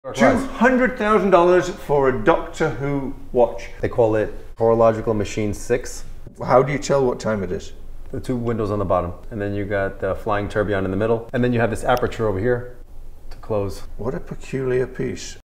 $200,000 for this weird watch!?!
This is an MB&F Spaceship watch, and it's as crazy as it is weird. Could you pull it off on your wrist? I'm not sure I could...